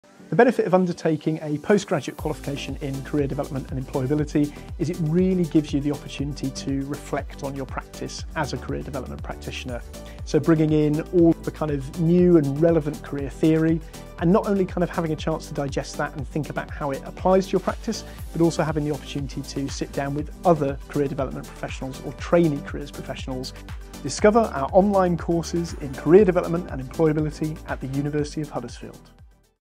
Career Development and Employability MA/PGDip – Benefits of Postgraduate Study
Watch our short video to discover the key benefits of studying Career Development and Employability at postgraduate level.